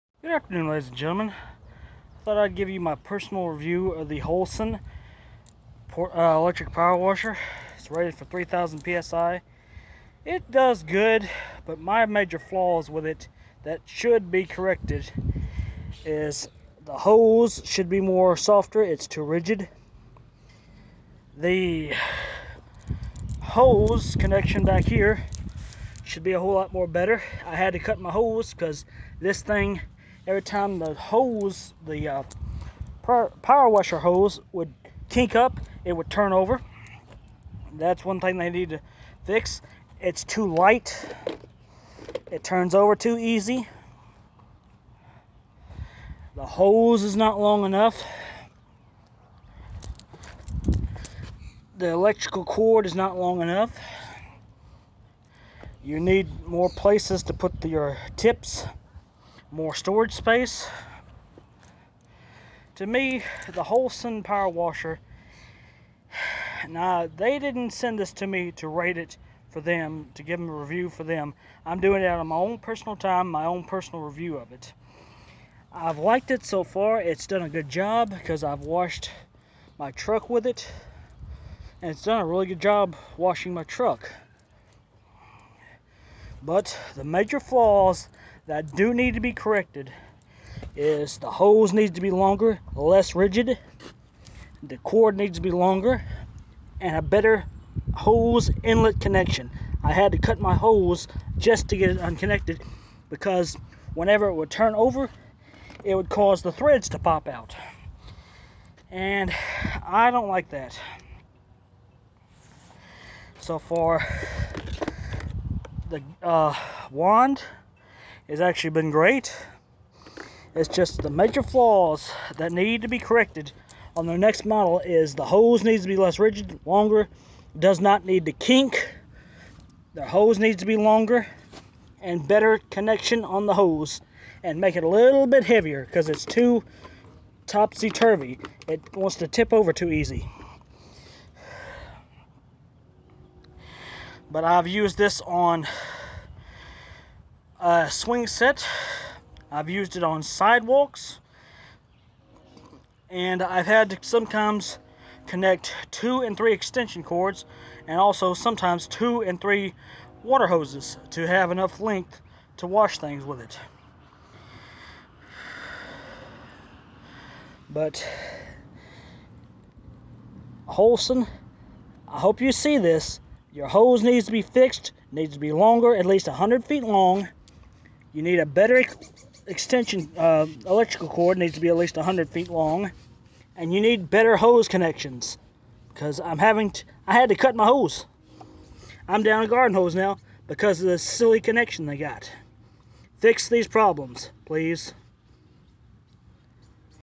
my personal review of the wholesun electric pressure washer
here is my personal review of the wholesun electric power washer the pros and the cons of this item